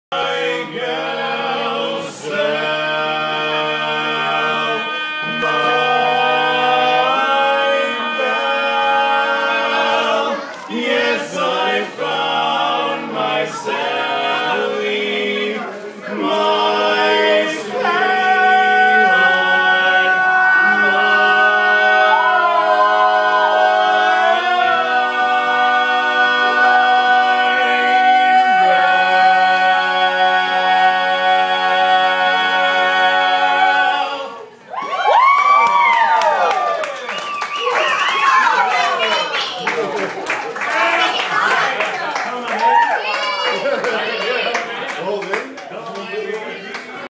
Barbershop Trio (part II)
Taylor, Franklin and Zach sing for Taylor's new bride, Heather!